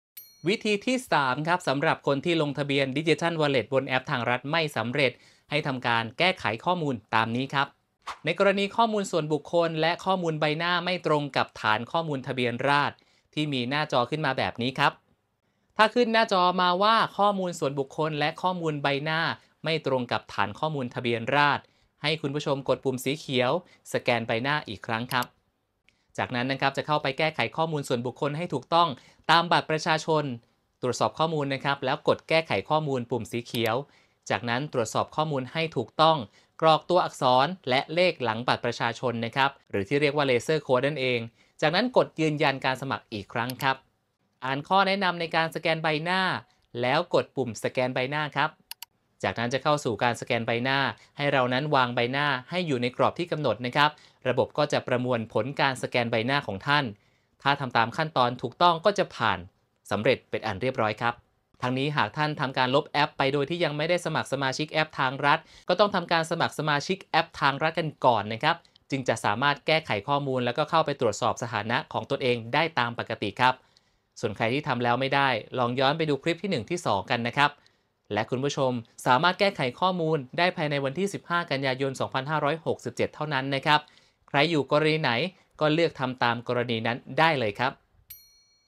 วิธีที่ 3 ข้อมูลส่วนบุคคลและข้อมูลใบหน้าไม่ตรงกับฐานข้อมูลทะเบียนราษฎร์ : เล่ามา อาร์ตี้ EP.83
วิธีที่ 3 ข้อมูลส่วนบุคคลและข้อมูลใบหน้าไม่ตรงกับฐานข้อมูลทะเบียนราษฎร์  ลงทะเบียนฯ Digital Wallet บนแอปฯ ทางรัฐ ไม่สำเร็จ ให้ทำการแก้ไขข้อมูลตามนี้